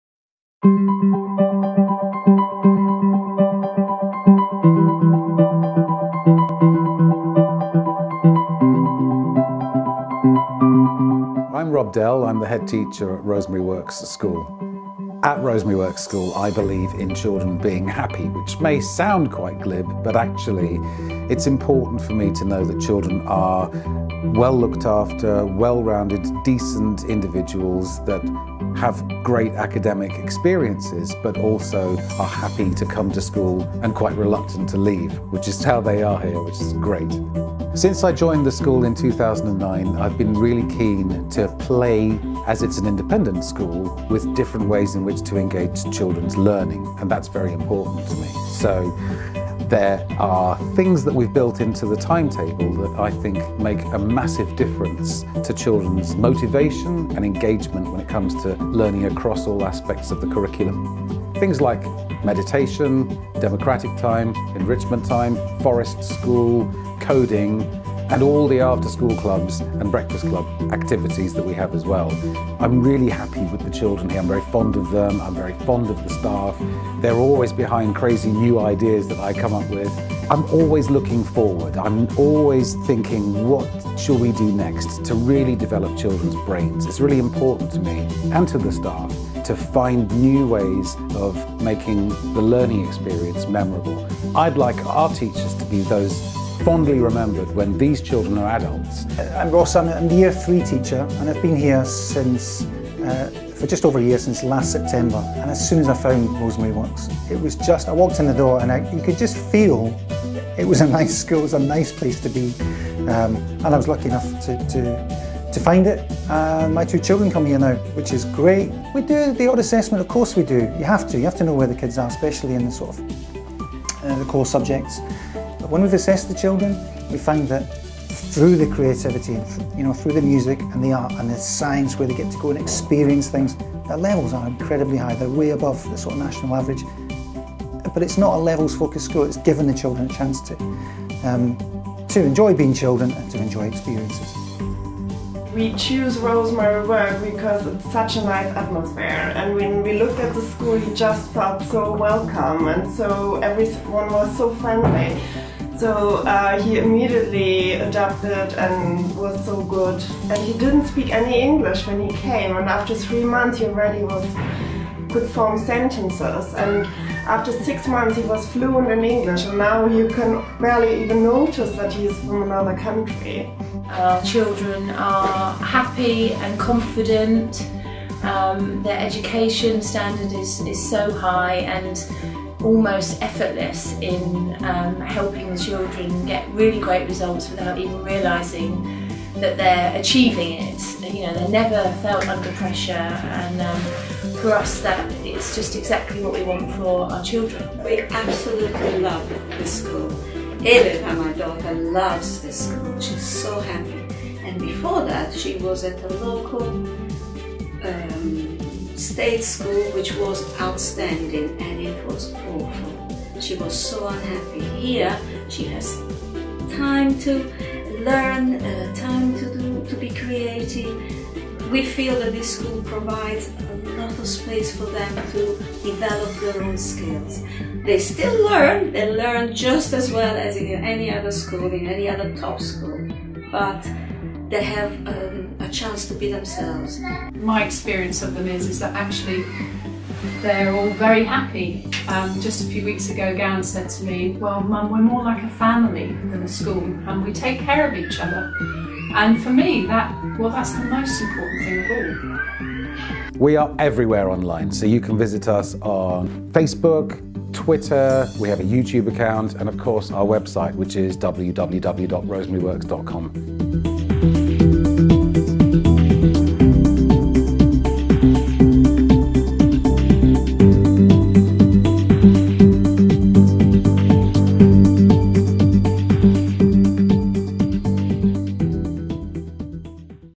Rosemary Works School
A brief introduction to the wonderful Rosemary Works School in Central London.

Special thanks to Rob Dell for additional footage.